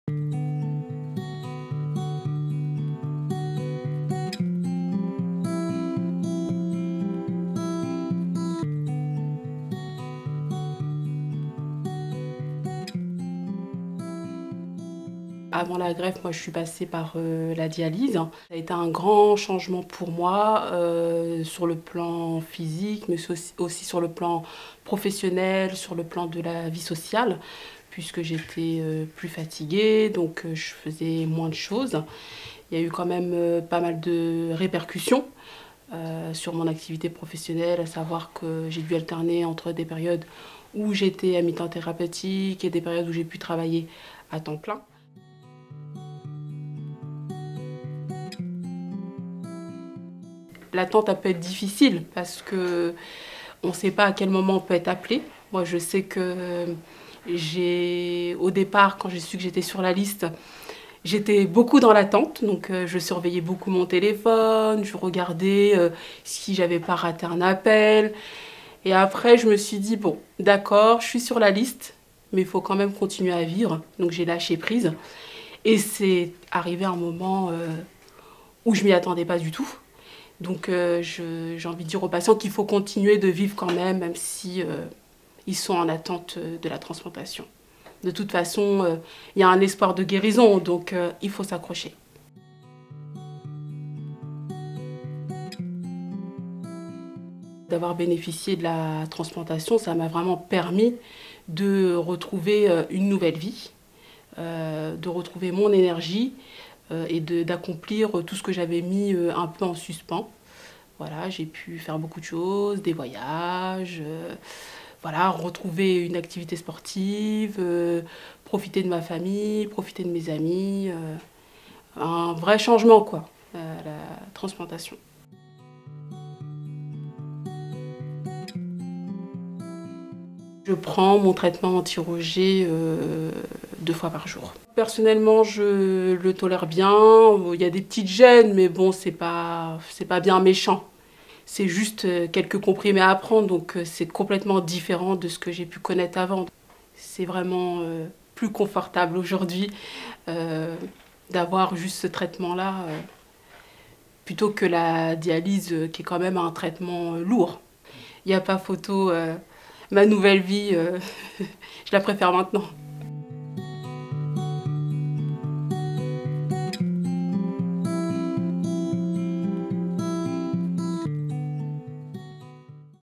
Témoignage de Kara
Kara a été dialysée 4 ans avant de bénéficier d'une greffe provenant d'un donneur décédé.